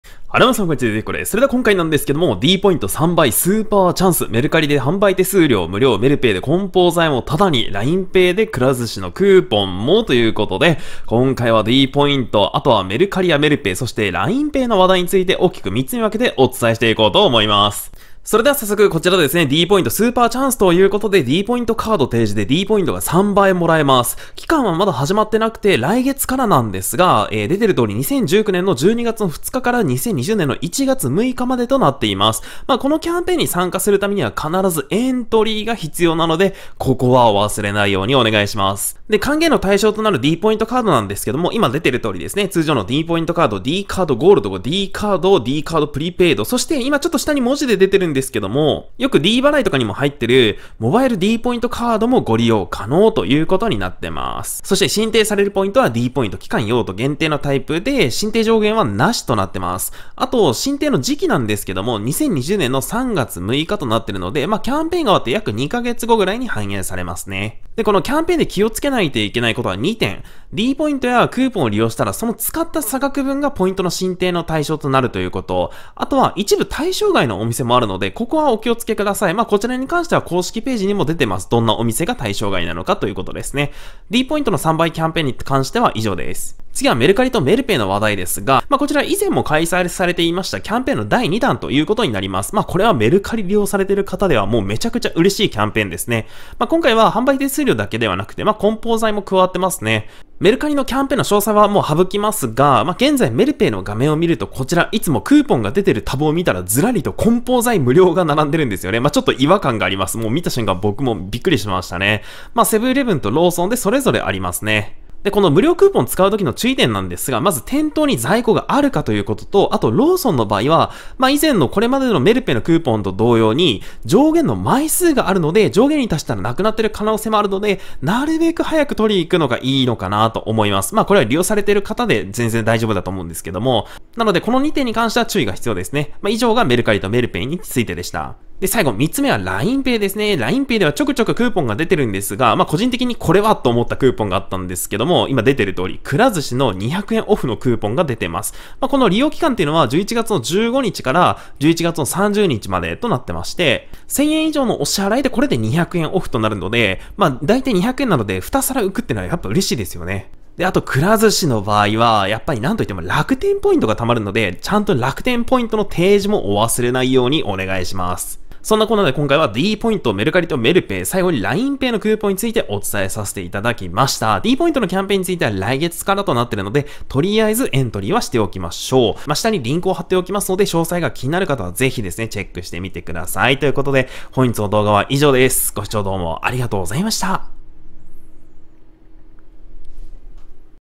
d払いで使えるdポイント3倍スーパーチャンス！メルカリで販売手数料無料メルペイで梱包材もタダに！LINEPayではくら寿司のクーポンも
【PR】 dポイントスーパーチャンス

使用機材
マイク：Rode NT1A
キャプチャーボード：I-O DATA GV-USB3
オーディオインターフェース：YAMAHA AG03